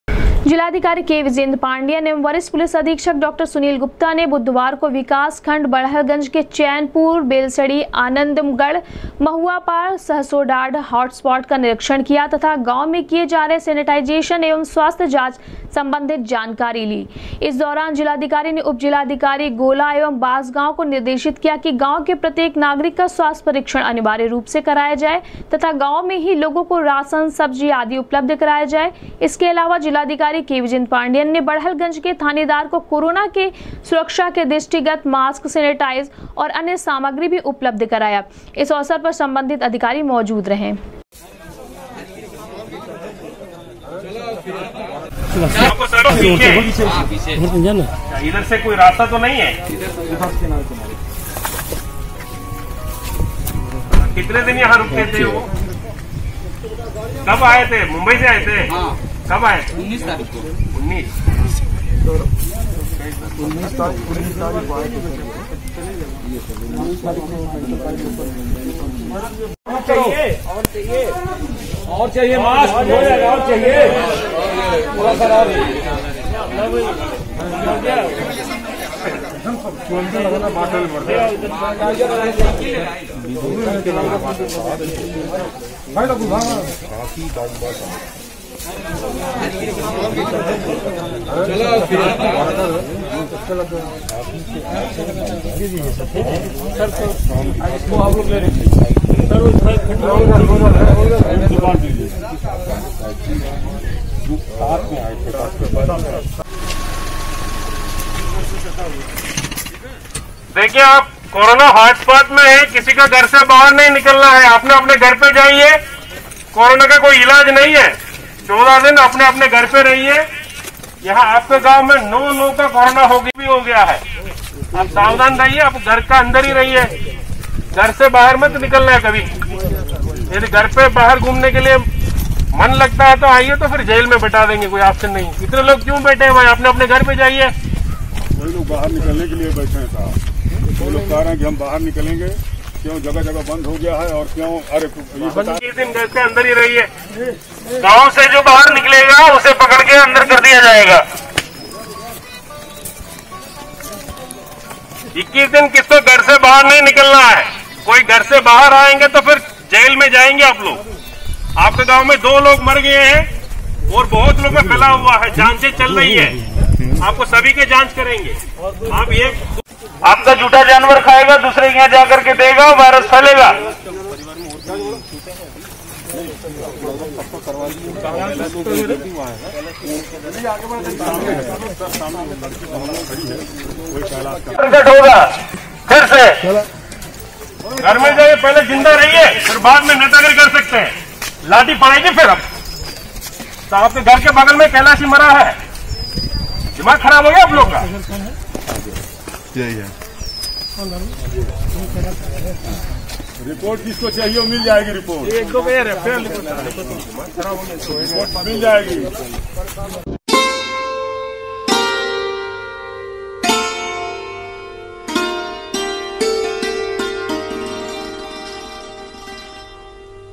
डीएम और एसएसपी ने किया बड़हलगंज के चैनपुर, बेलसड़ी, आनन्दगढ़, महुआपार, सहसों डाड़
डीएम और एसएसपी ने किया बड़हलगंज के चैनपुर, बेलसड़ी, आनन्दगढ़, महुआपार, सहसों डाड़ हाटस्पाट का निरीक्षण, सभी के स्वास्थ्य जांच के दिये निर्देश।।।। GORAKHPUR NEWS -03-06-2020